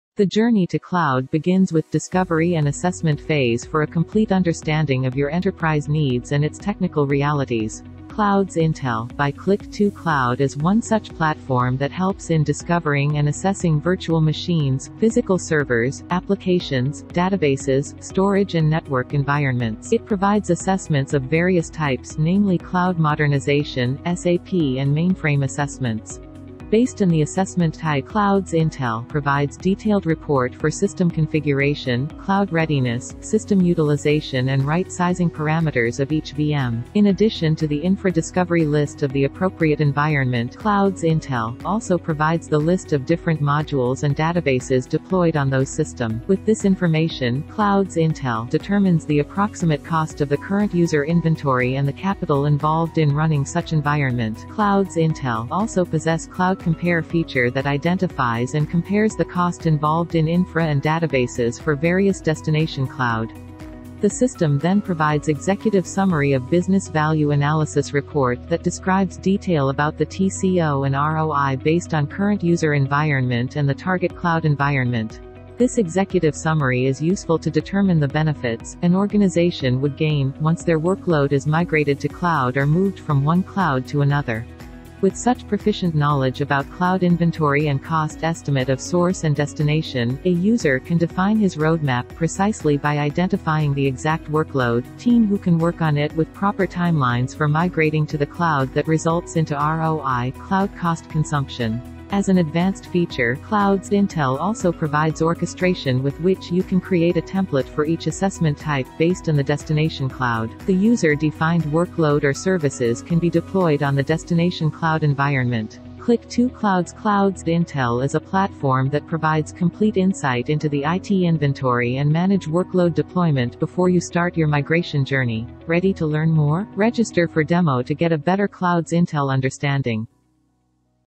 Click2Cloud-Launching Cloud Intel-An Assessment platform
Discover your IT inventories with Clouds Intel to make informed decisions on your migration activities.